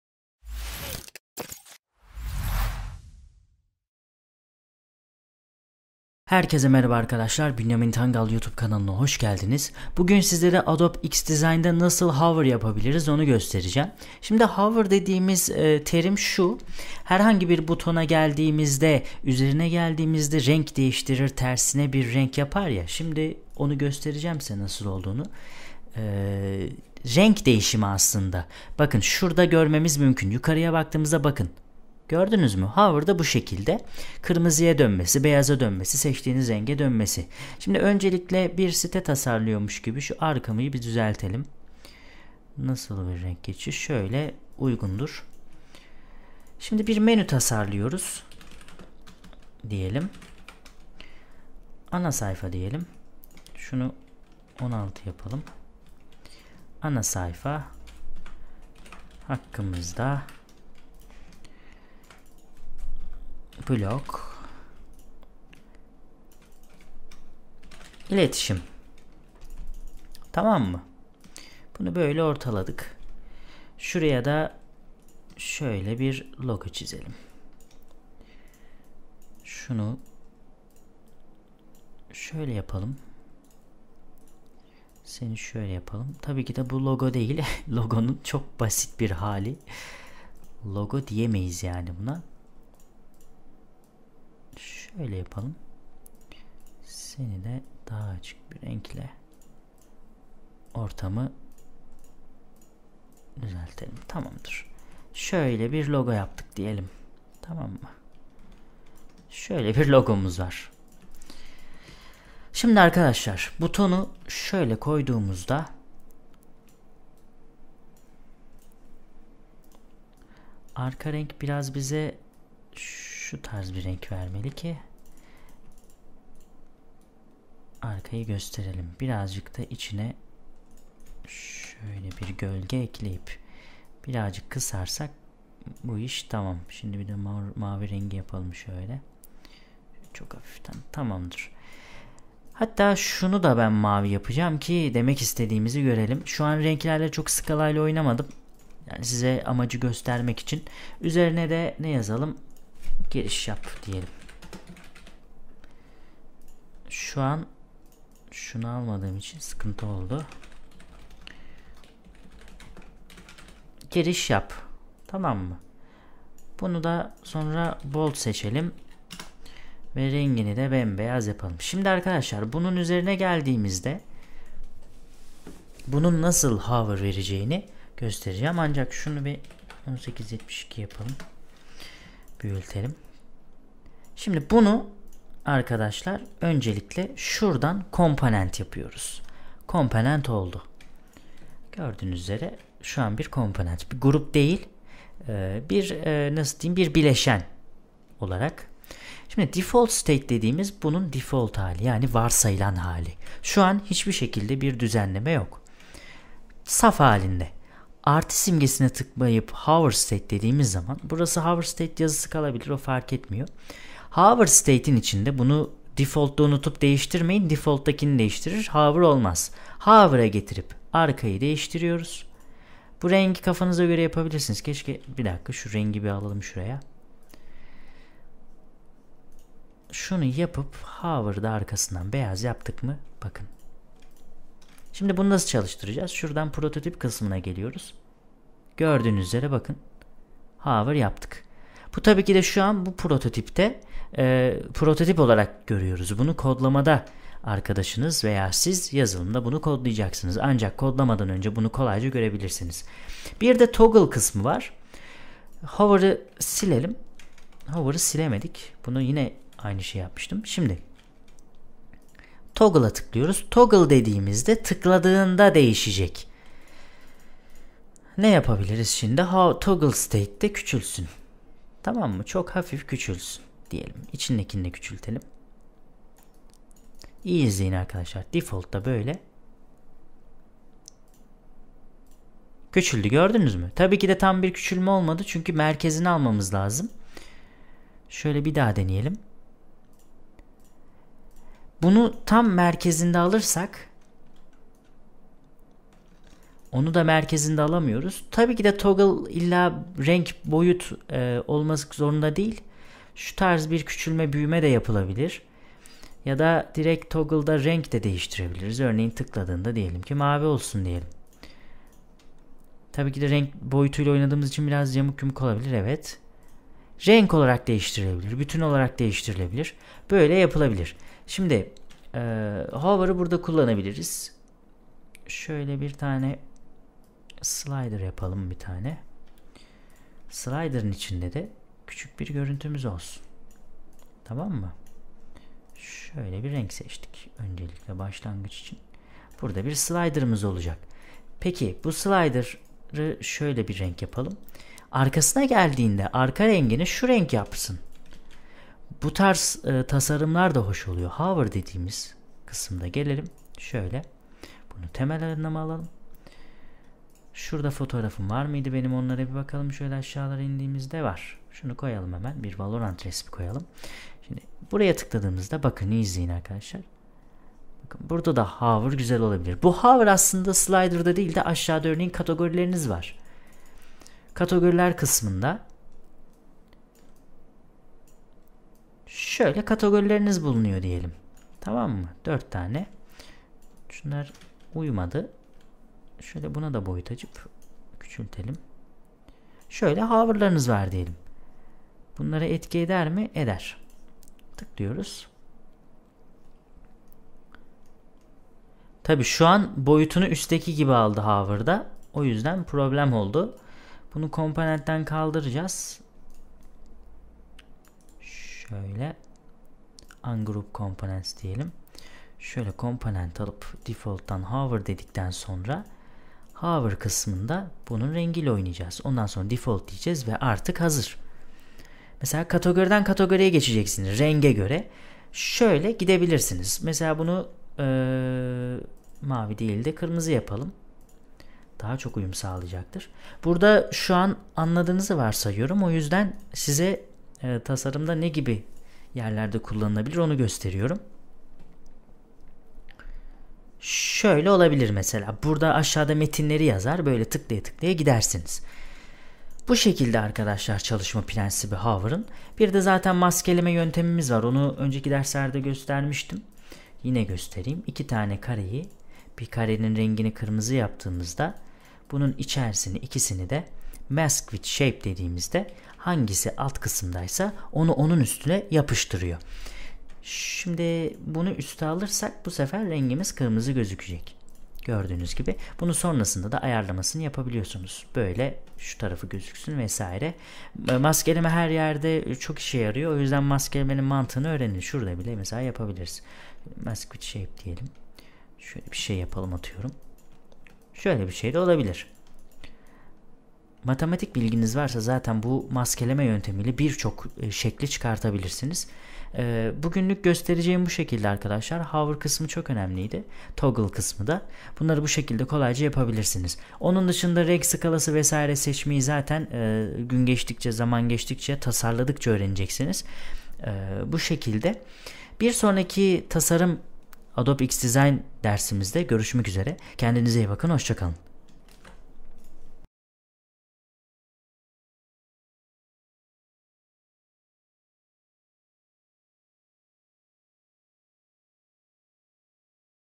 adobe xd ile hover nasıl yapılır
merhaba arkadaşlar bu gün adobe xd ile hover nasıl yapılır sizlere gösterecem iyi seyirler.